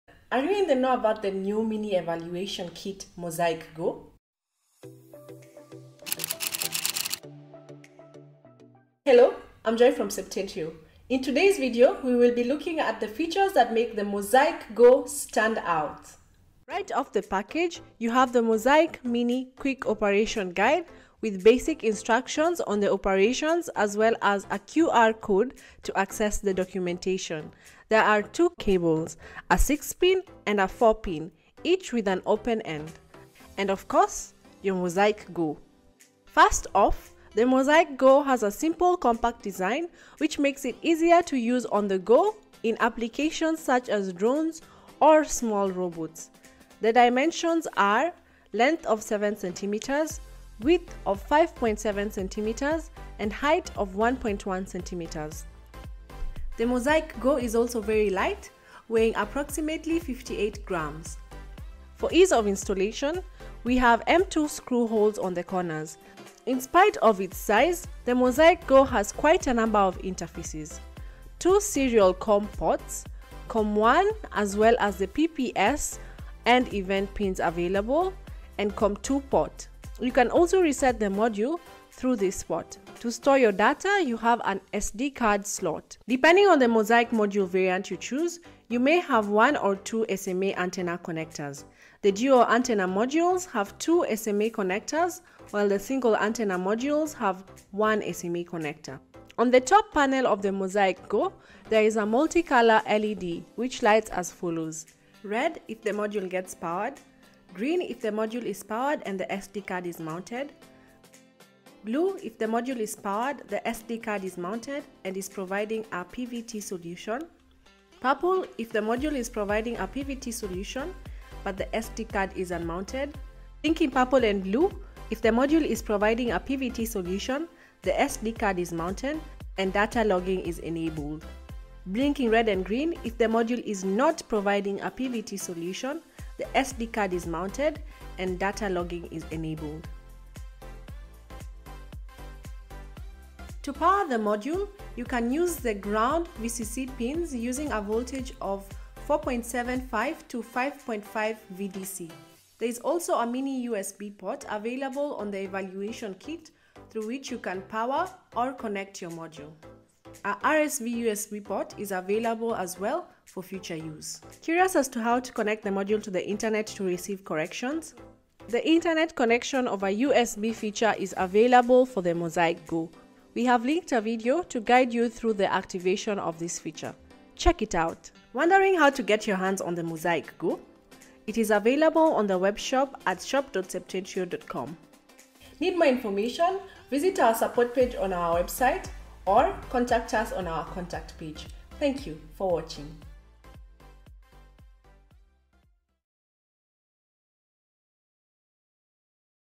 How to use Septentrio's mosaic-go GNSS module evaluation kit
Septentrio's mosaic-go is small, light in weight and has quite a number of interfaces. Learn more about the features that make the evaluation kit easy to use on the go. Find out how to use it in this video.The mosaic-go is available for purchase on shop.septentrio.com.